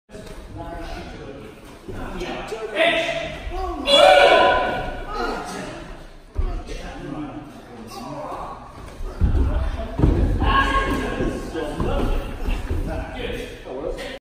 Tekki Sandan Bunkai
A quick look at some bunkai to Tekki Sandan, helping our students to learn the opening sequence